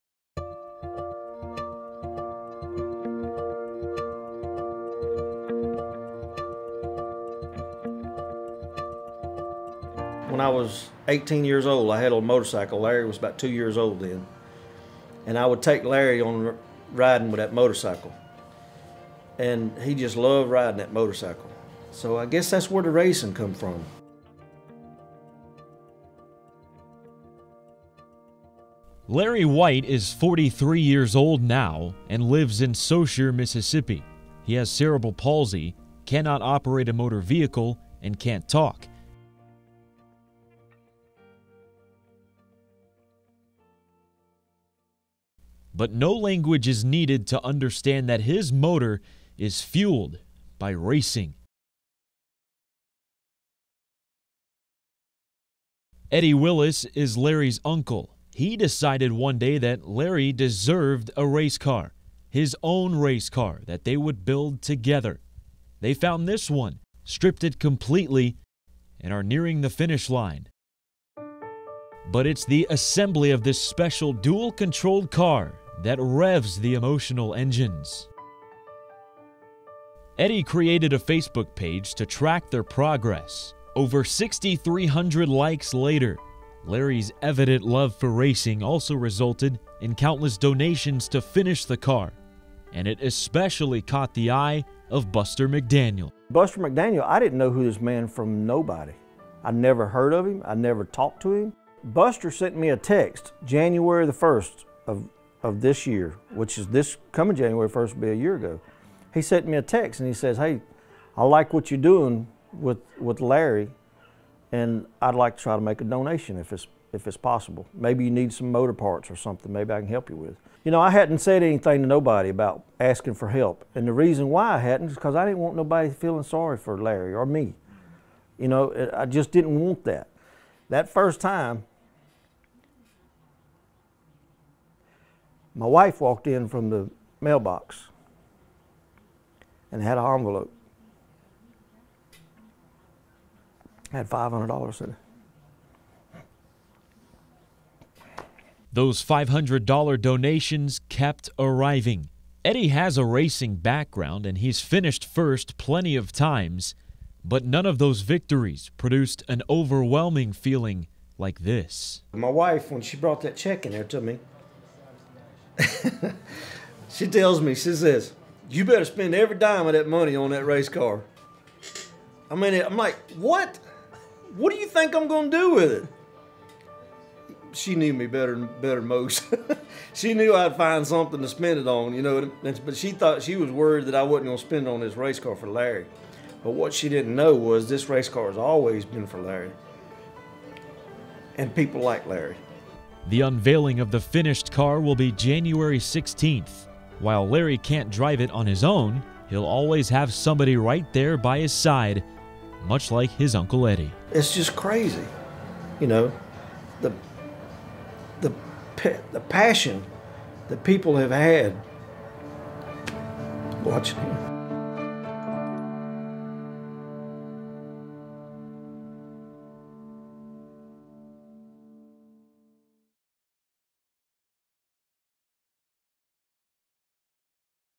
WLOX - LARRY WHITE PROJECT 12/20/2015
Tyler Bouldin explains how donations and overwhelming support helped one man build a race car for his nephew.